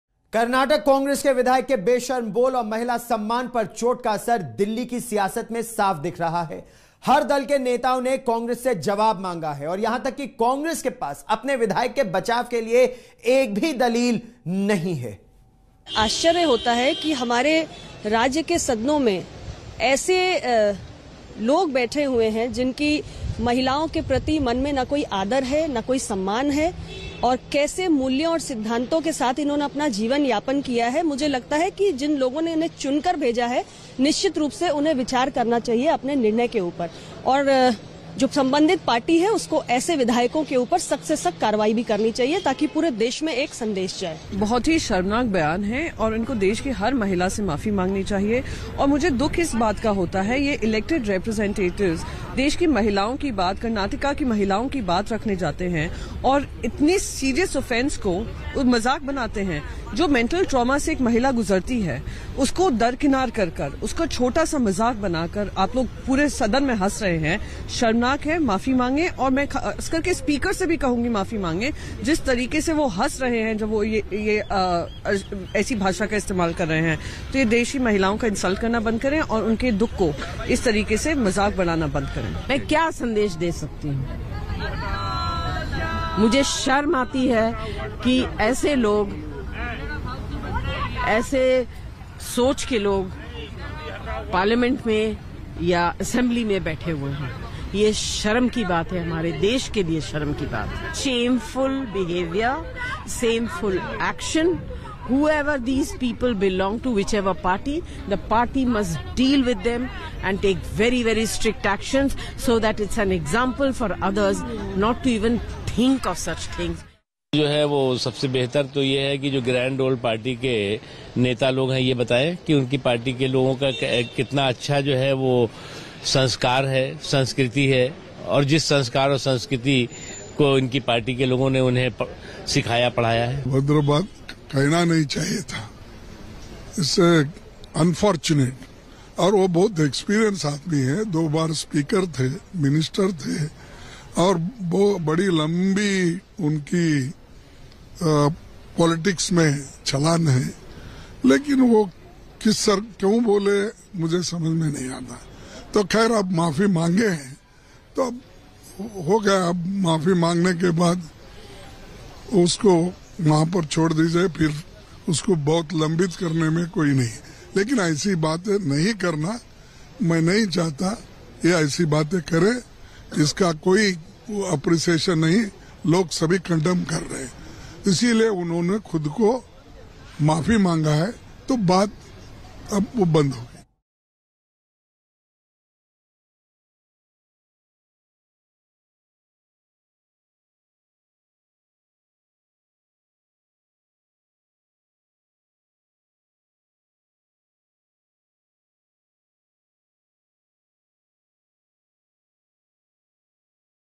Congress MLA Ramesh ने मांगी माफी, दुर्भाग्य है उन्होंने रेप को लेकर ऐसा कहा: Mallikarjun Kharge
Karnataka Congress MLA Ramesh Kumar के विवादित बयान ने एक बार फिर लोकतंत्र की गरिमा को शर्मसार कर दिया है. इस पर कांग्रेस नेता Mallikarjun Kharge ने कहा कि ये काफी दुर्भाग्यपूर्ण है. उन्होंने कहा कि आखिर Ramesh Kumar जी ने ऐसा क्यों कहा, ये तो नहीं पता.